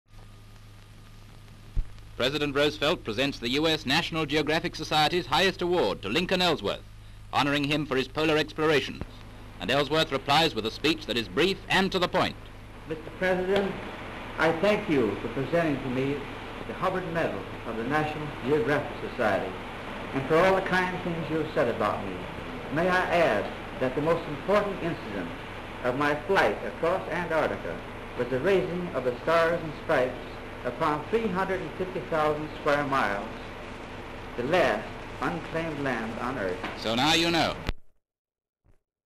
Washington (1936)
Full title reads: "Washington".

Washington DC, United States of America (USA).

VS American President Franklin D Roosevelt presents National Geographic Society's Hubbard medal to Antarctic Explorer Lincoln Ellsworth in the White House.

(Sound continues after end of pictures).

N.B. Item found in Unidentified Gazettes reel. May be missing item from 36/35.
 FILM ID:2957.21